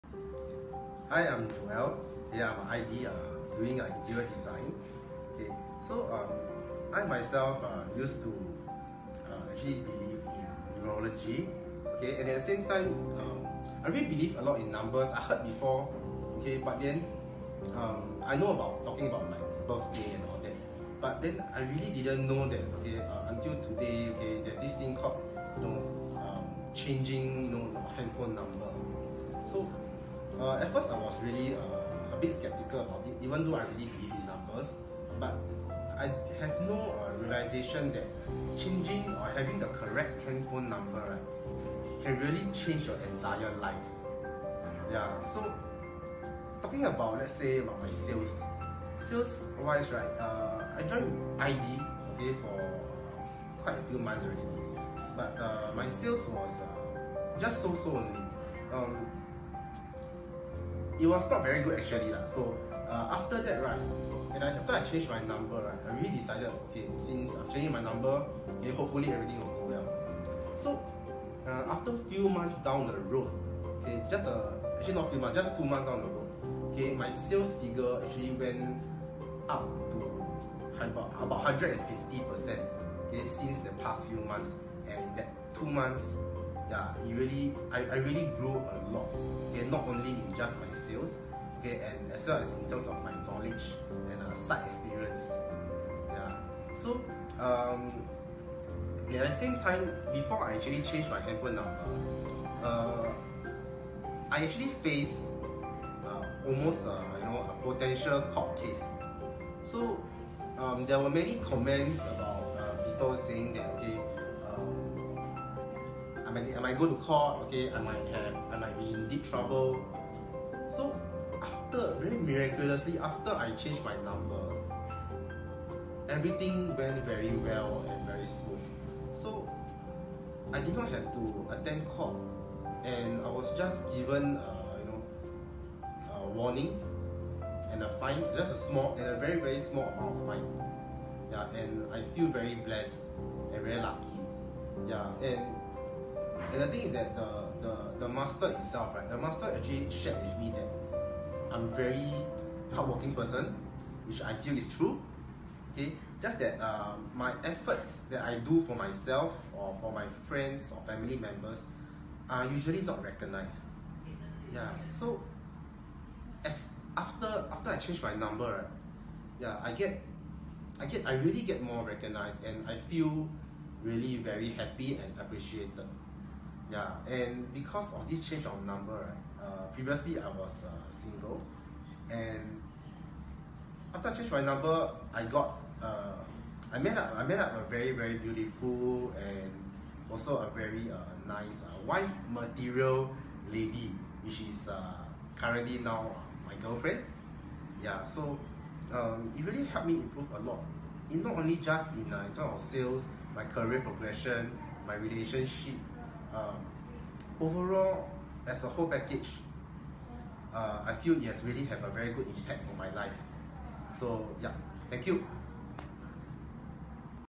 COSMOMK: Joel Chin Testimonial | Upgrade Numbers Thankful 141319
Thank you Joel for sharing with us your journey after upgrading your numbers with CosmoMK!